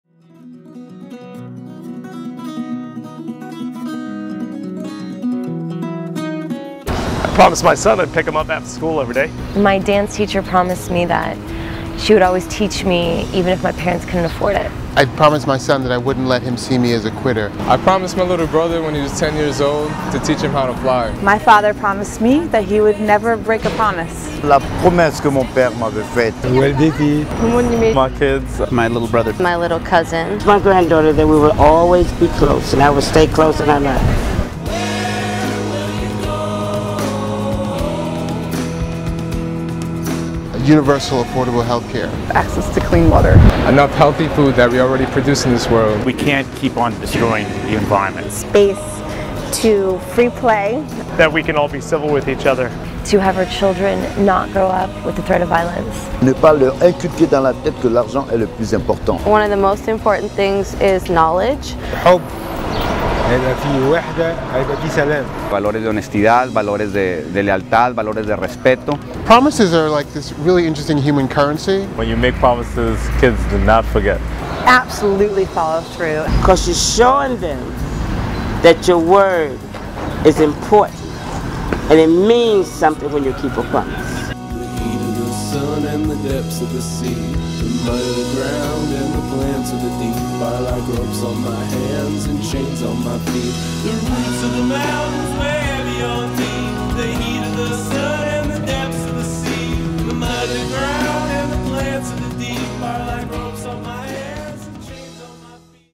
The Value of a Promise
We are asking people to share their stories about promises for children. We invite you to use the digital recorder form on UNICEF's global Facebook or on the microsite By sharing your story, you will encourage others to join the global conversation on child survival.

A compilation of videos will be put together to display some of the collective promises to the world's children, in the run up to this September's meeting of the General Assembly.

Please spread the word, via your social networks, with your colleagues, friends and families!

Background:
On June 14, Committing to Child Survival: A Promise Renewed will be launched. By pledging to support the goals of A Promise Renewed, partners vow to redouble efforts and to focus on reaching the most disadvantaged and hardest-to-reach children in every country.

We'd like you to join us and add your voice to the global commitment. Tell us -- What is your promise to the children of the world?

Directed by Tim Gregory